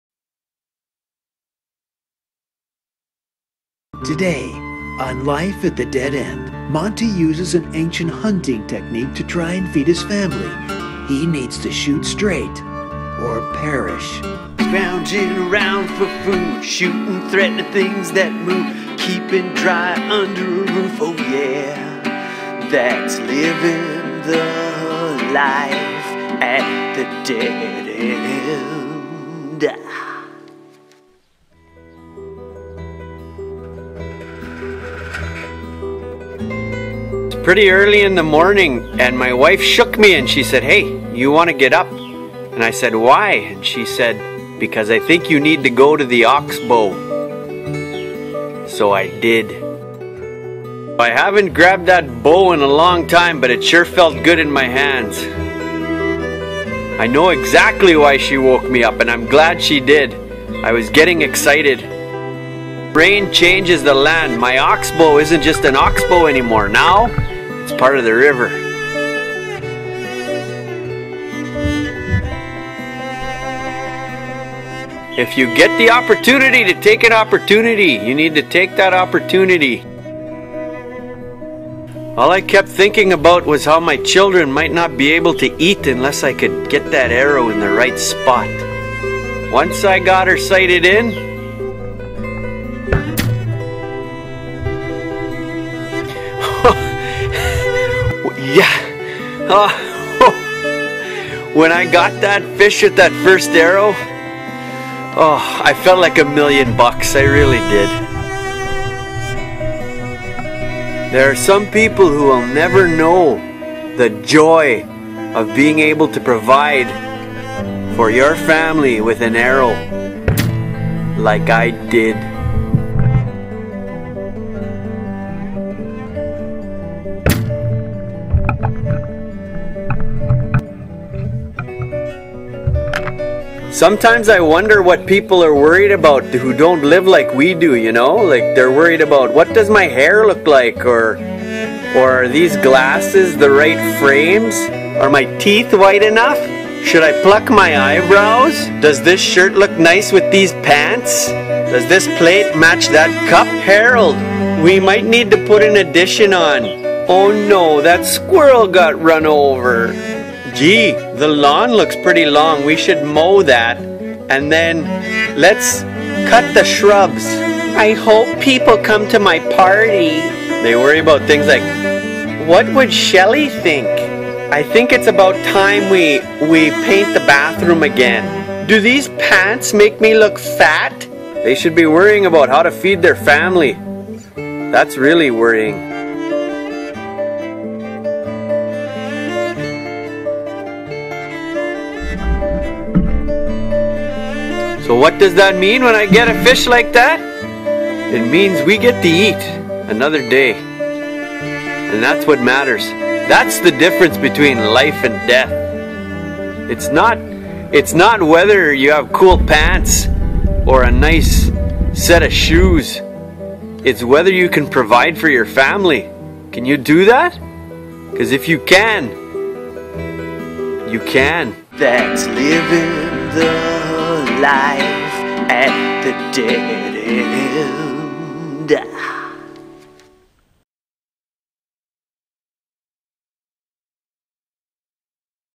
Life at the Dead End: Carp eh DIE 'em, Episode 8
Monte's wife alerts him to an opportunity to forage for food in flooding waters. He aims...literally...to keep his family alive. Carp fish is on the menu. Will he succeed or flounder?